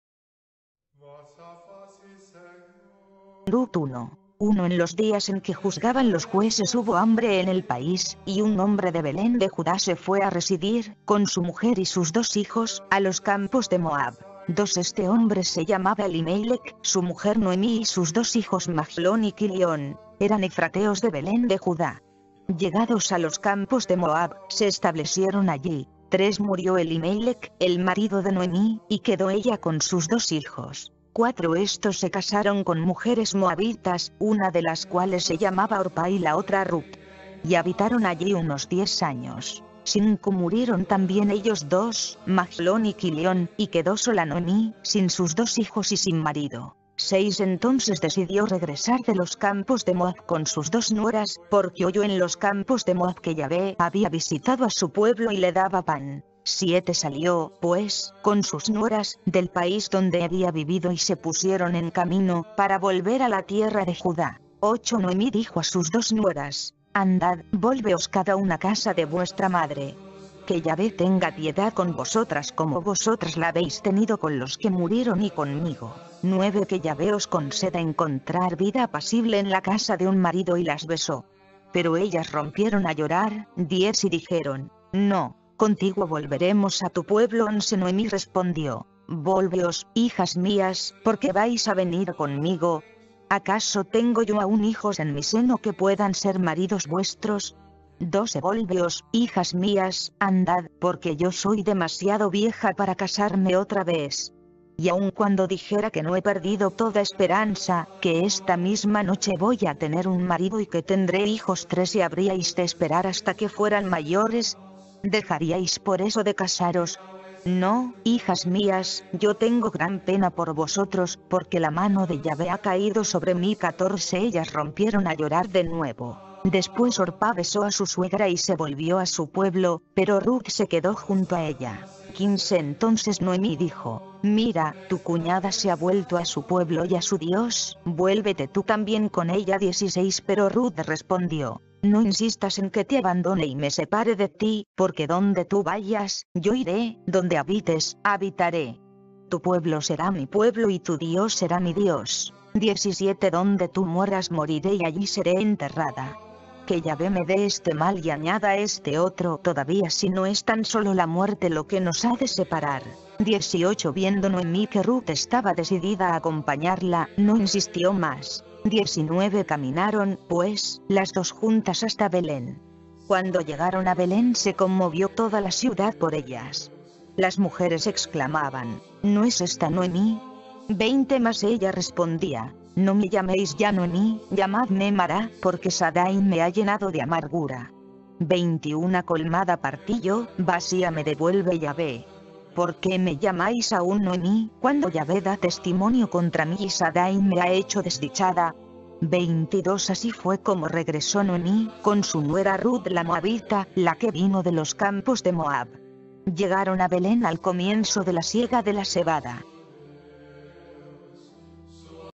LA SANTA BIBLIA,VERSIÓN BIBLIA DE JERUSALÉN 1976, Rut 1
Rut 1
1 En los días en que juzgaban los Jueces hubo hambre en el país, y un
hombre de Belén de Judá se fue a residir, con su mujer y sus dos hijos, a los
campos de Moab.
2 Este hombre se llamaba Elimélek, su mujer Noemí y sus dos hijos
Majlón y Kilyón; eran efrateos de Belén de Judá. Llegados a los campos de
Moab, se establecieron allí.
3 Murió Elimélek, el marido de Noemí, y quedó ella con sus dos hijos.
4 Estos se casaron con mujeres moabitas, una de las cuales se llamaba
Orpá y la otra Rut. Y habitaron allí unos diez años.
5 Murieron también ellos dos, Majlón y Kilyón, y quedó sola Noemí,
sin sus dos hijos y sin marido.
6 Entonces decidió regresar de los campos de Moab con sus dos
nueras, porque oyó en los campos de Moab que Yahveh había visitado a su
pueblo y le daba pan.
7 Salió, pues, con sus nueras, del país donde había vivido y se
pusieron en camino, para volver a la tierra de Judá.
8 Noemí dijo a sus dos nueras: «Andad, volveos cada una a casa de
vuestra madre. Que Yahveh tenga piedad con vosotras como vosotras la
habéis tenido con los que murieron y conmigo.
9 Que Yahveh os conceda encontrar vida apacible en la casa de un
marido.» Y las besó. Pero ellas rompieron a llorar,
10 y dijeron: «No; contigo volveremos a tu pueblo.»
11 Noemí respondió: «Volveos, hijas mías, ¿por qué vais a venir
conmigo? ¿Acaso tengo yo aún hijos en mi seno que puedan ser maridos
vuestros?
12 Volveos, hijas mías, andad, porque yo soy demasiado vieja para
casarme otra vez. Y aun cuando dijera que no he perdido toda esperanza,
que esta misma noche voy a tener un marido y que tendré hijos
13 ¿habríais de esperar hasta que fueran mayores? ¿dejaríais por eso
de casaros? No, hijas mías, yo tengo gran pena por vosotros, porque la
mano de Yahveh ha caído sobre mí.»
14 Ellas rompieron a llorar de nuevo; después Orpá besó a su suegra y
se volvió a su pueblo, pero Rut se quedó junto a ella.
15 Entonces Noemí dijo: «Mira, tu cuñada se ha vuelto a su pueblo y a
su dios, vuélvete tú también con ella.»
16 Pero Rut respondió: «No insistas en que te abandone y me separe
de ti, porque donde tú vayas, yo iré, donde habites, habitaré. Tu pueblo será
mi pueblo y tu Dios será mi Dios.
17 Donde tú mueras moriré y allí seré enterrada. Que Yahveh me dé
este mal y añada este otro todavía si no es tan sólo la muerte lo que nos ha
de separar.»
18 Viendo Noemí que Rut estaba decidida a acompañarla, no insistió
más.
19 Caminaron, pues, las dos juntas hasta Belén. Cuando llegaron a
Belén se conmovió toda la ciudad por ellas. Las mujeres exclamaban: «¿No
es esta Noemí?»
20 Mas ella respondía: «¡No me llaméis ya Noemí, llamadme Mará,
porque Sadday me ha llenado de amargura!
21 Colmada partí yo, vacía me devuelve Yahveh. ¿Por qué me llamáis
aún Noemí, cuando Yahveh da testimonio contra mí y Sadday me ha hecho
desdichada?»
22 Así fue como regresó Noemí, con su nuera Rut la moabita, la que
vino de los campos de Moab. Llegaron a Belén al comienzo de la siega de
la cebada.